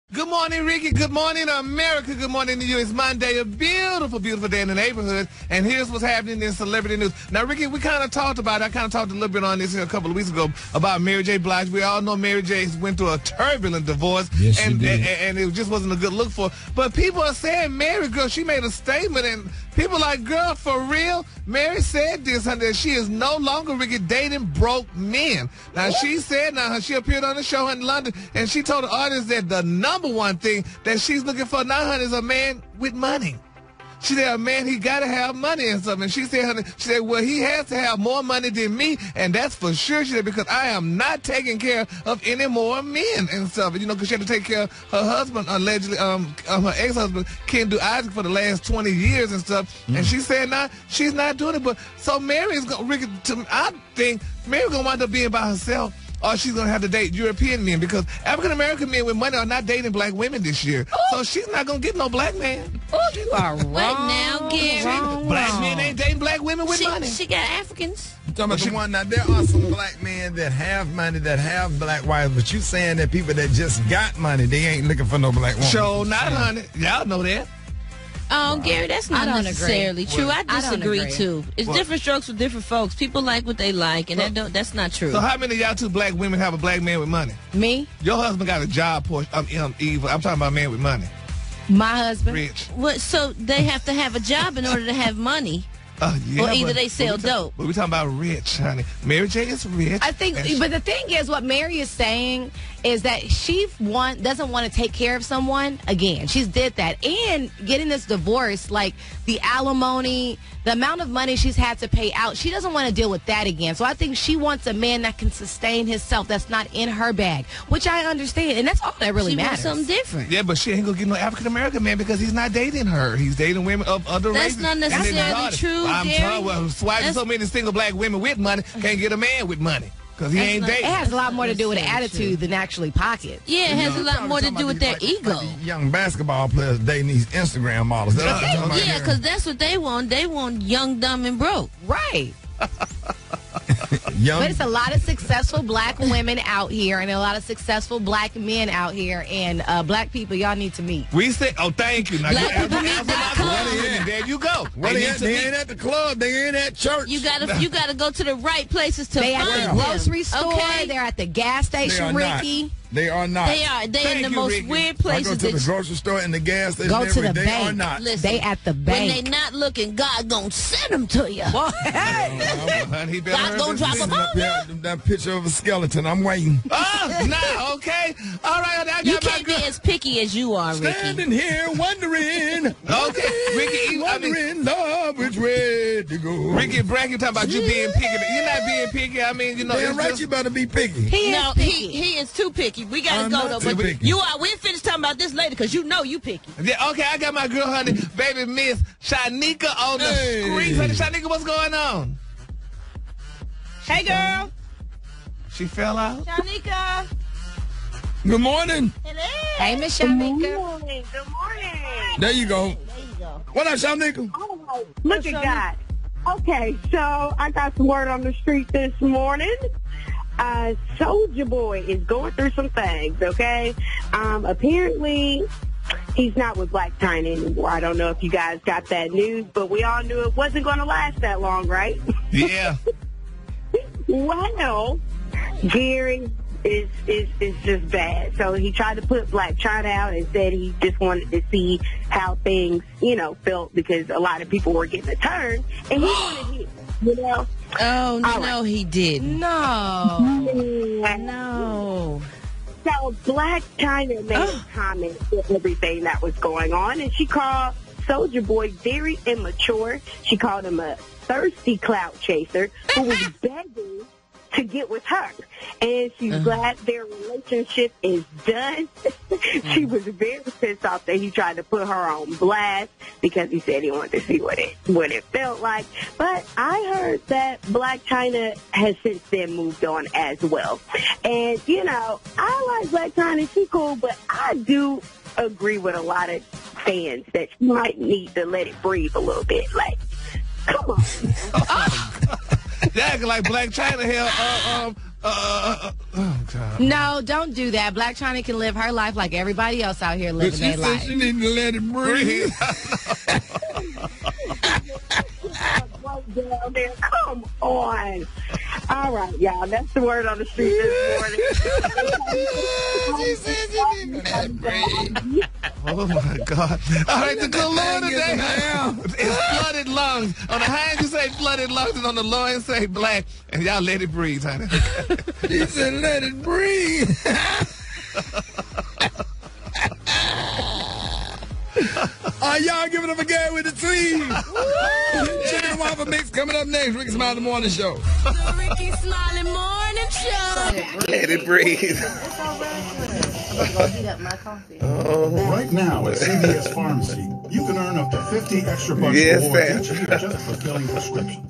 Why Mary J. Blige Probably Won't Find A Man With Money
In this edition of Gary's Tea, I talk about why Mary J. Blige wants a man with money, but probably won't get one. Plus, my girl Mz. Shyneka from Hot 107.9 in Atlanta gives the latest on Soulja Boy and Blac Chyna.